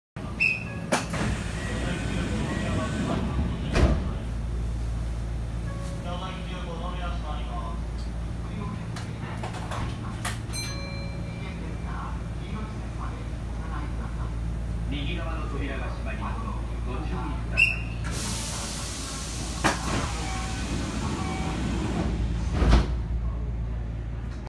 【近鉄1233系】左右で違うドア開閉音
2019/09/03 に撮影
今まで全く気が付かなかったのですが、左右でドアの開閉音が違うようです。
両開きだったこともあり、開く時に気づいたので、閉じる時に撮影しました♪

【夏休み本州周遊大遠征】